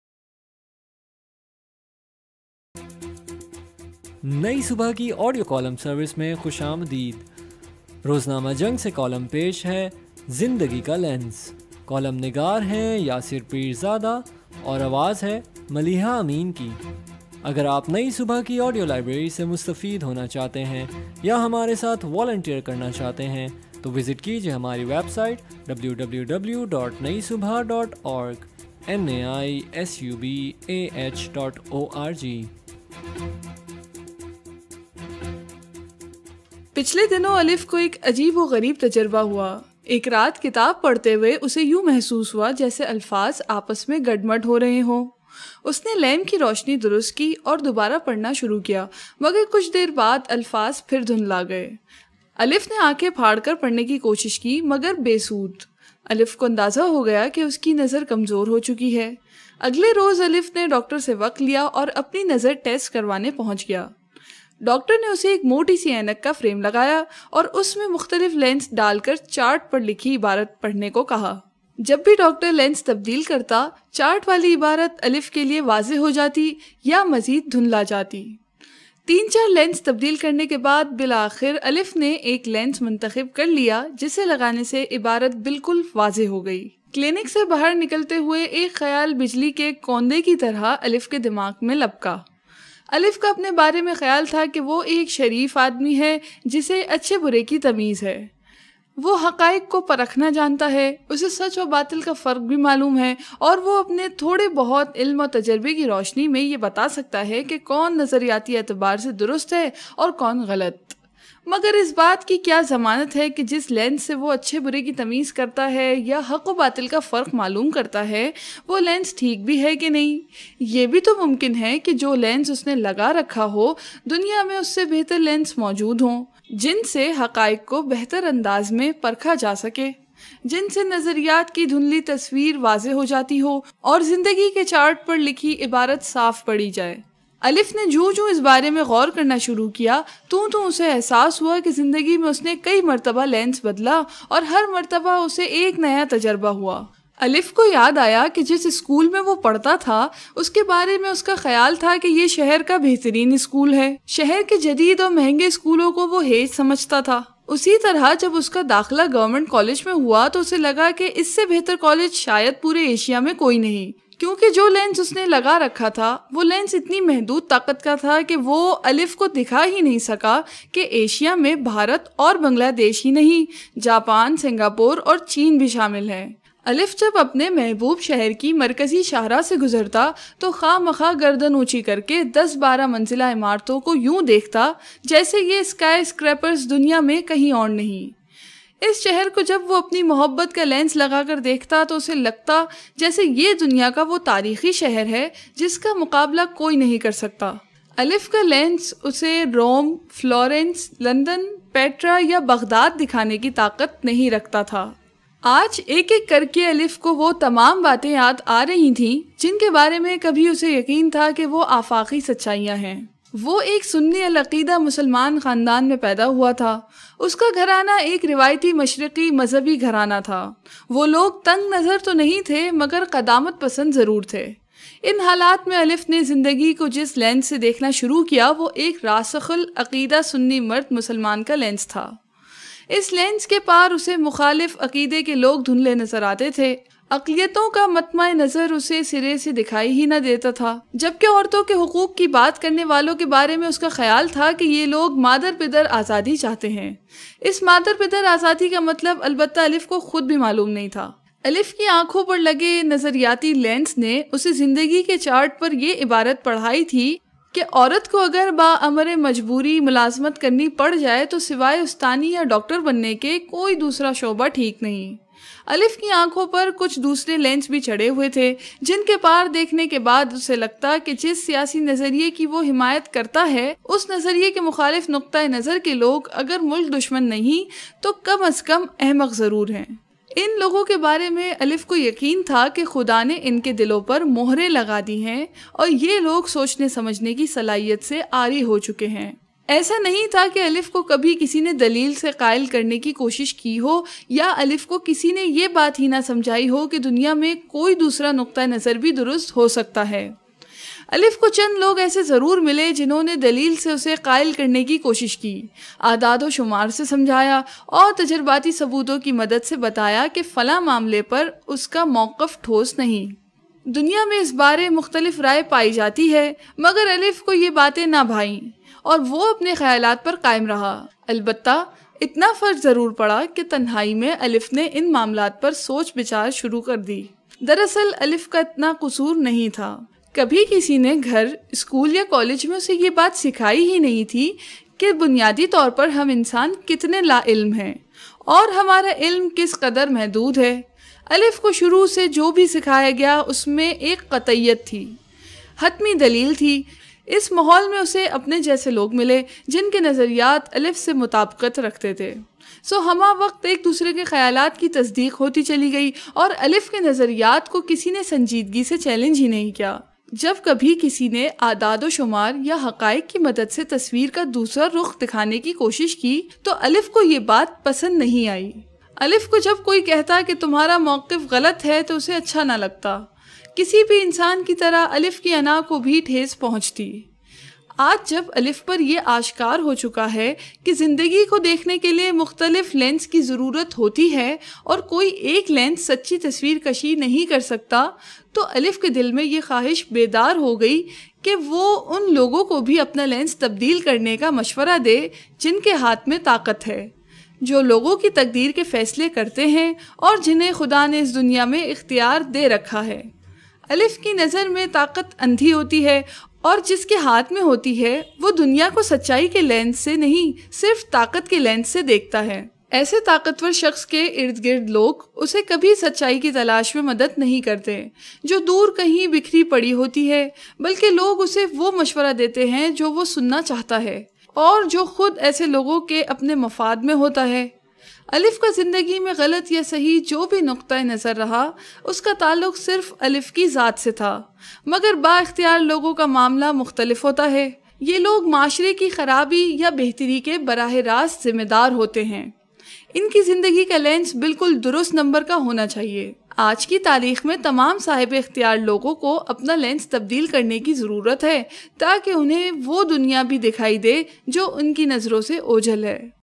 Zindagi ka lens by Yasir Peerzada
Nai Subah is an organization working on the capacity building of youth with disabilities. Audio column service has been initiated to facilitate people with visual impairment.